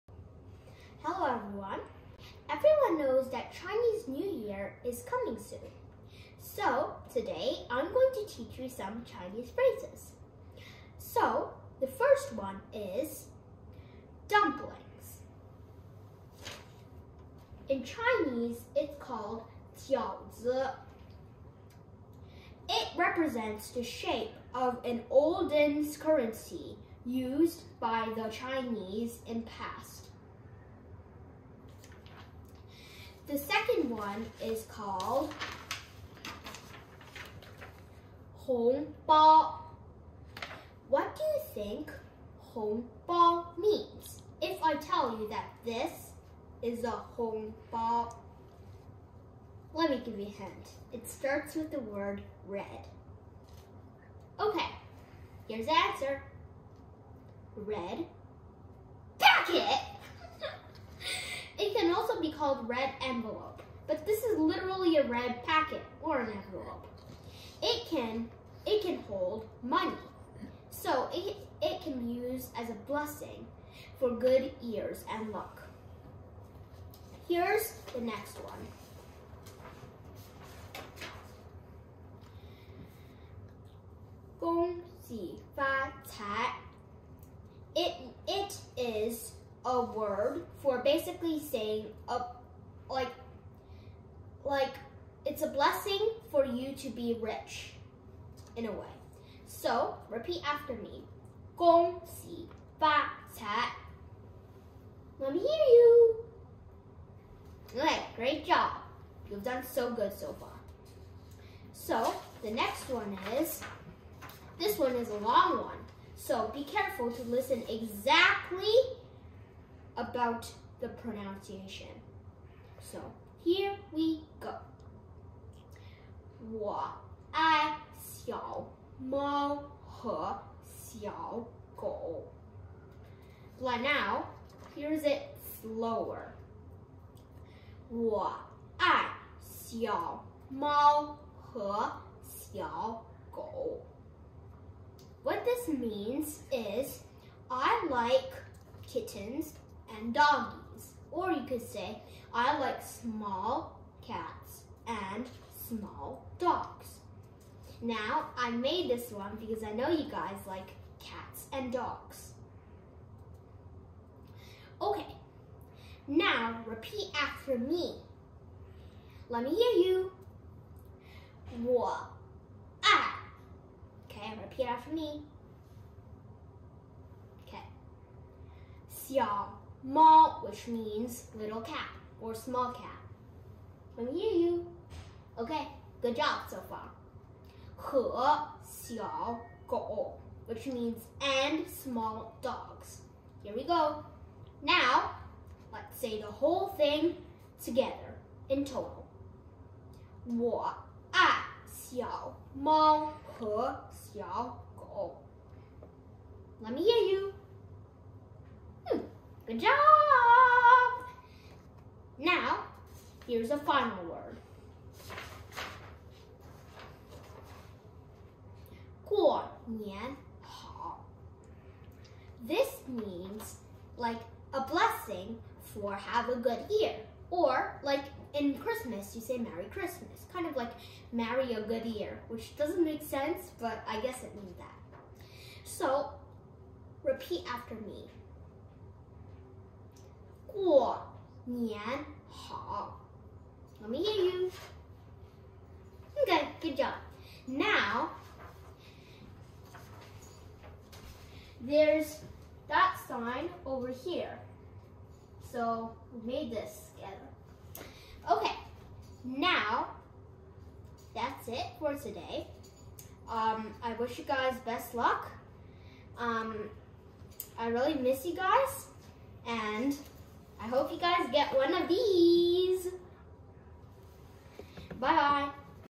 Happy Spring Festival! Chinese New Year phrases or words 2022
Jacob 8 years old 新春快乐! xīn chūn kuài lè

Happy Chinese New Year! 新春快乐!

In this video, you'll learn the 5 most popular Chinese New Year phrases or words for 2022.

Do you know how to say a happy new year in Mandarin?

Wish you all a Happy New Year and success in your Chinese studies.
May all your wishes come true, and best wishes for the year of the Tiger.